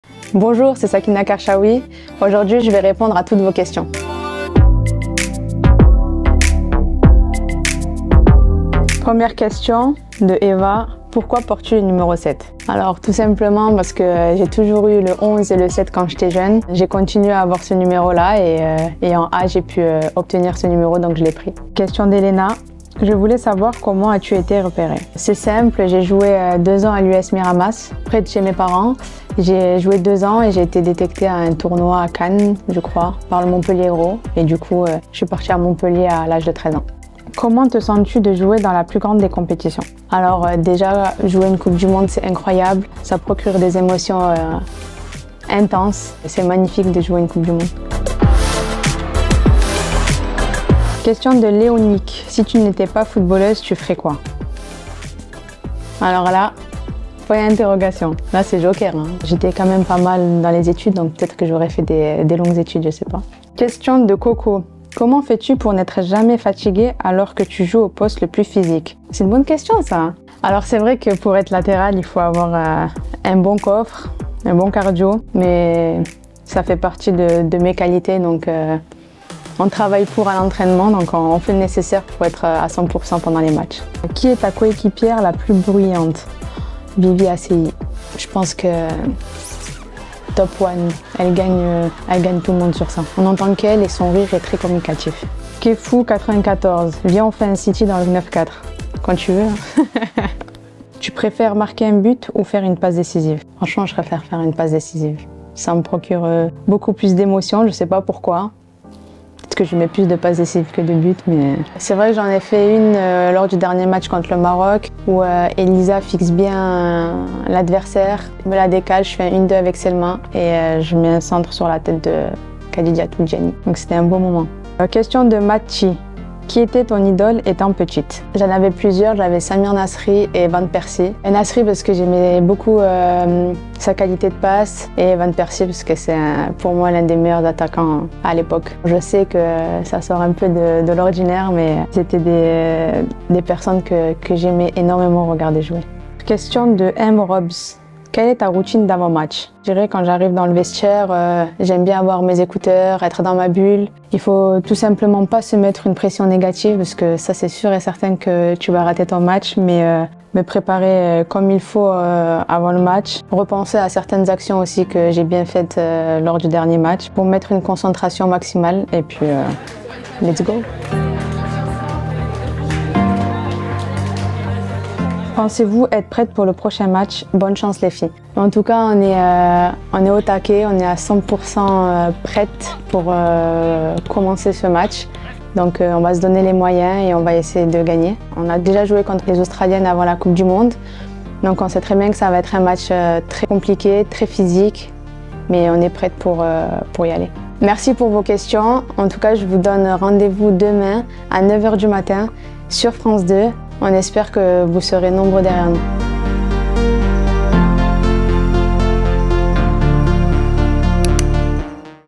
Sakina Karchaoui répond à vos questions ! CDM#47 I FFF 2023
À la veille de France - Australie, la défenseure des Bleues Sakina Karchaoui répond aux questions que vous lui avez posé sur Instagram à l'occasion d'une story spéciale sur le compte @equipedefrancef